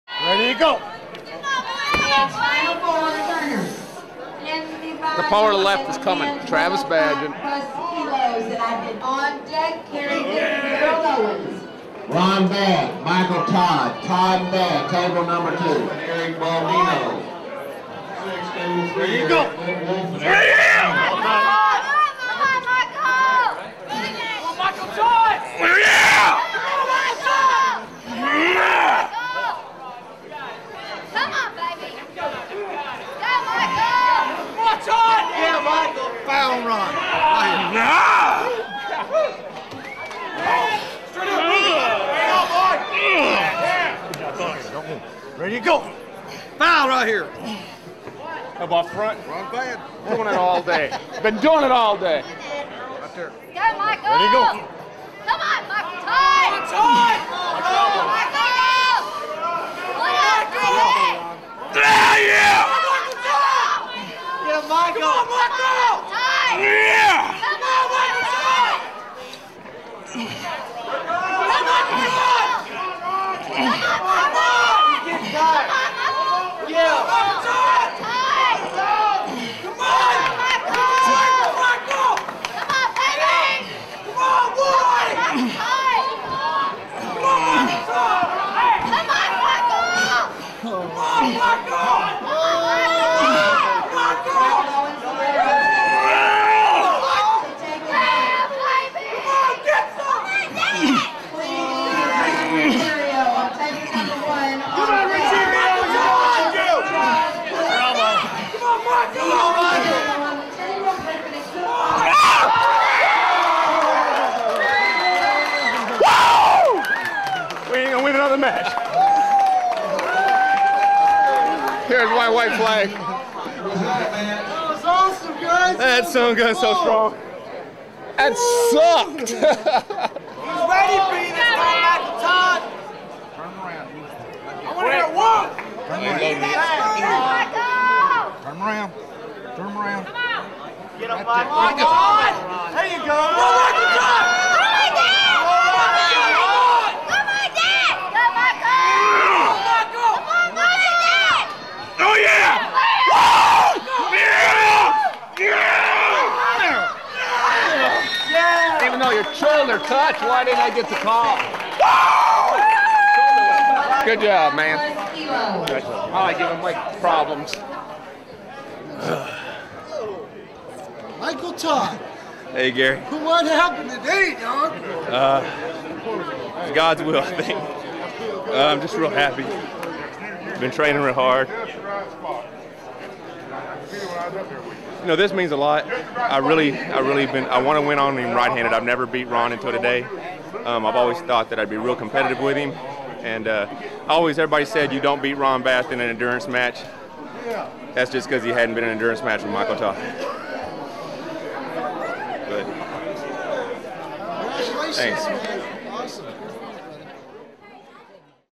First big win of Michael Todd's armwrestling career (added second match)
I did an interview with Michael Todd way back in the day and he told me this was the biggest win of his career. I know he has had some special ones since but I think this first big win still holds a special place in his heart. You can always see the exact moment Michael knows.

(AAA Nationals, 2005, Left hand)